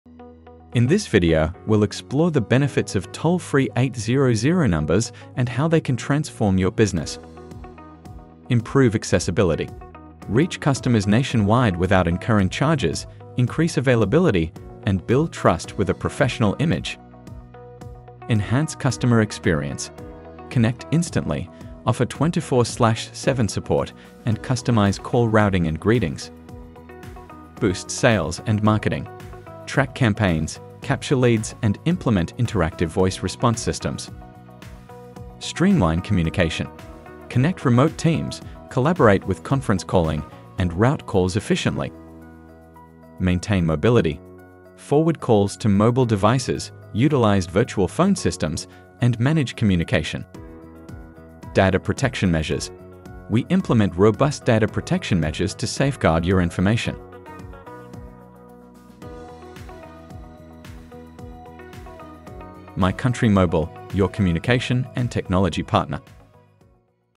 800 Area Code Toll-Free-Number l My Country Mobile
800 Area Code Toll-Free-Number | My Country Mobile : Discover the power of the 800 Area Code Toll-Free-Number in our comprehensive video. This toll-free number is the epitome of accessibility, enabling businesses to enhance customer service and expand their reach.